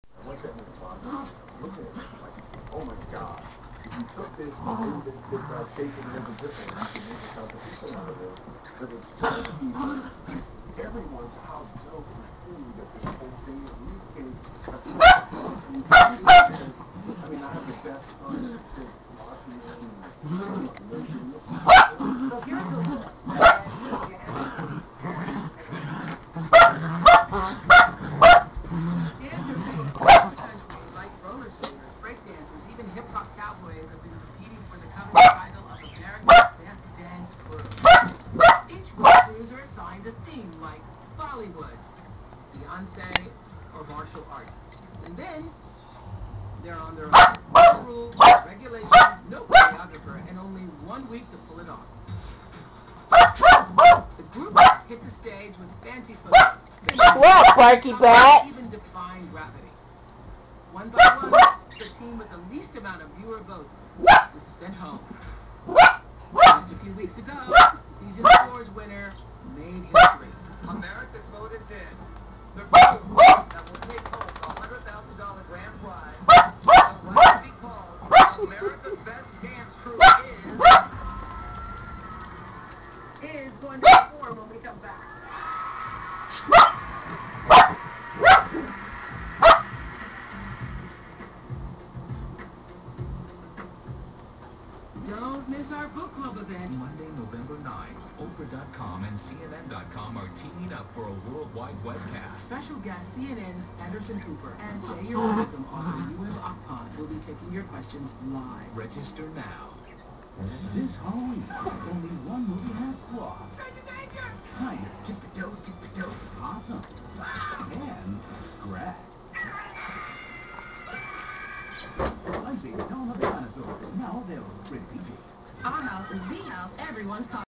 Wiener wrestling featuring Ms. Barky Butt
Beatrix, Lacey and Mira wrestling around